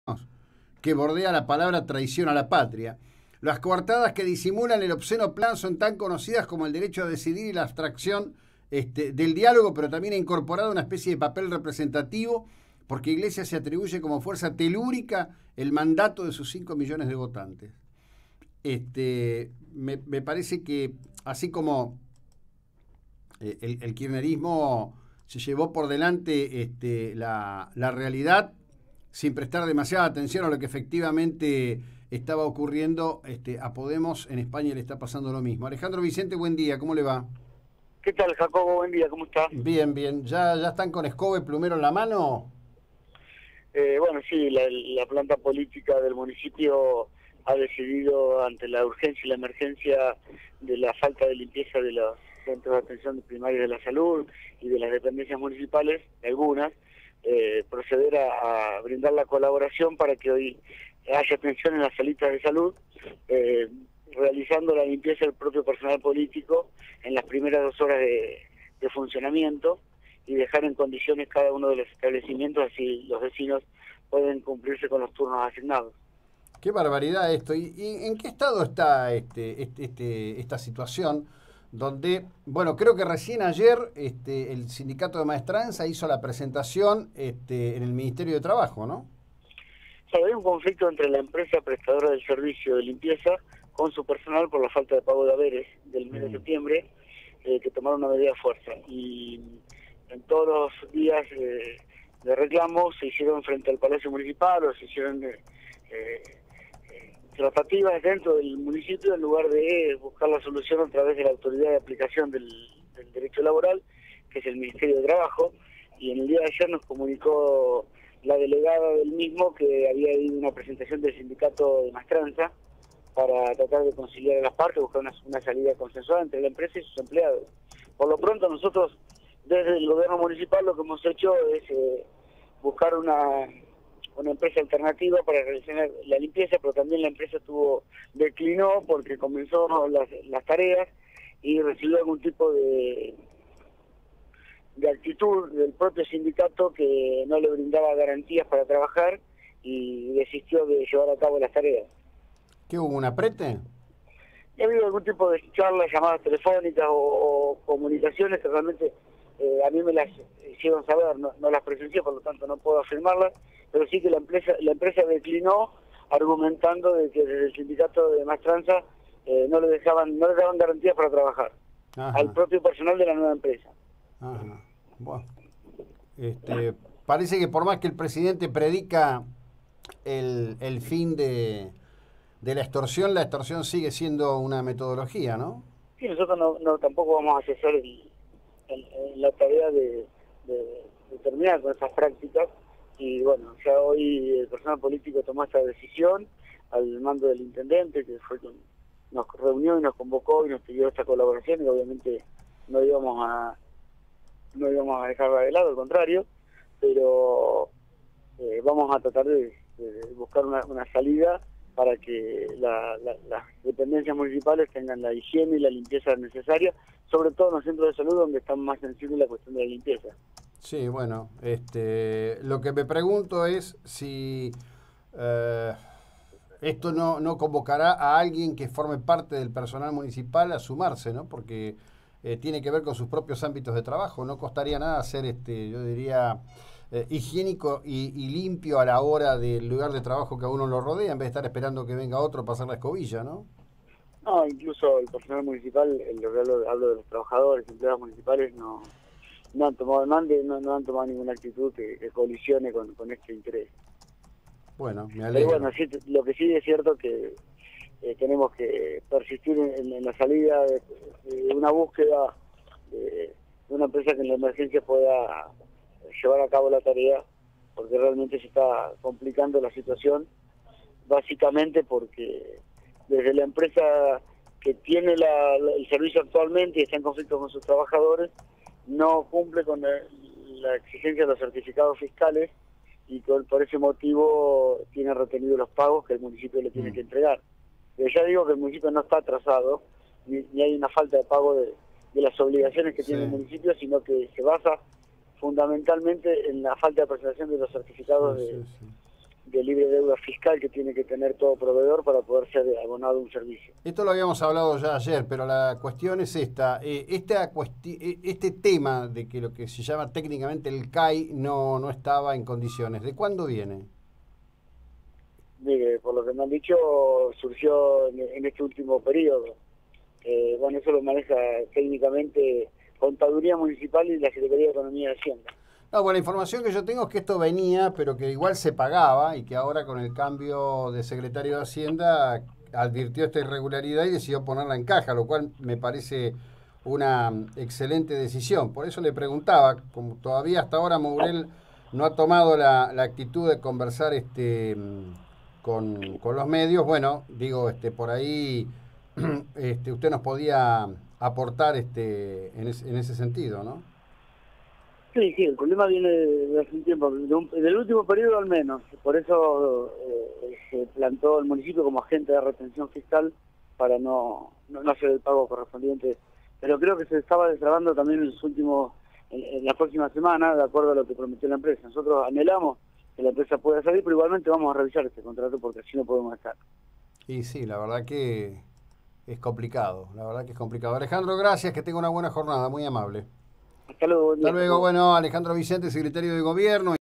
ALEJANDRO VICENTE 13-10-2017
El Secretario de Gobierno señaló que la planta política del municipio decidió hacerse cargo de la limpieza de los centros de salud.